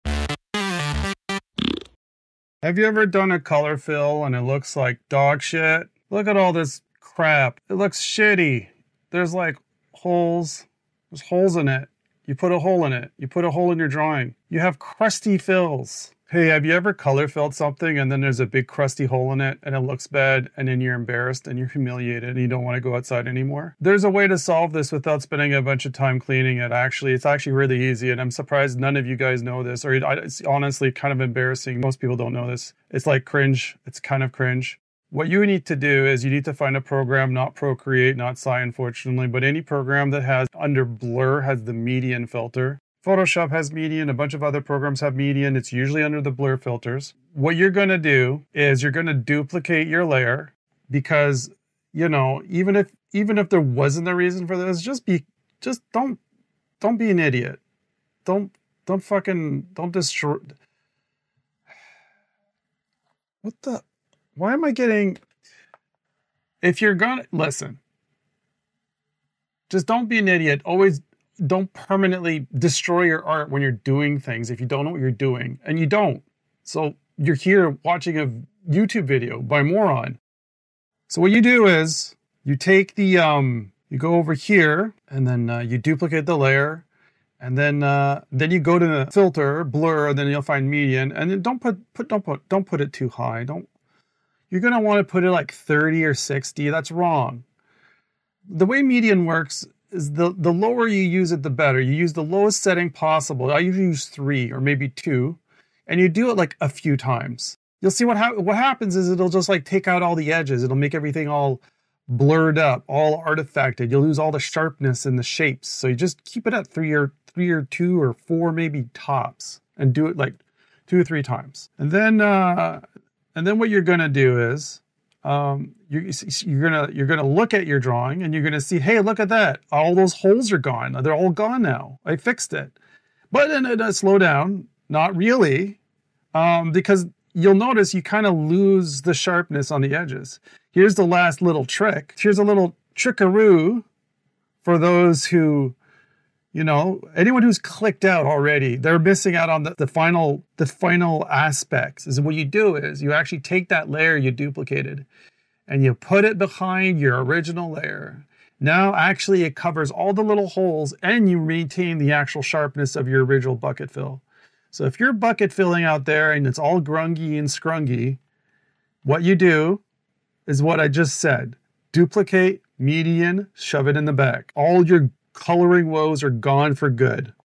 Fix your color fills stopsuckingnow!
Don't you hate it when you suck at color fills and your color fills are filled with garbage holes. WELL NO MORE. WITH THIS simple little trick.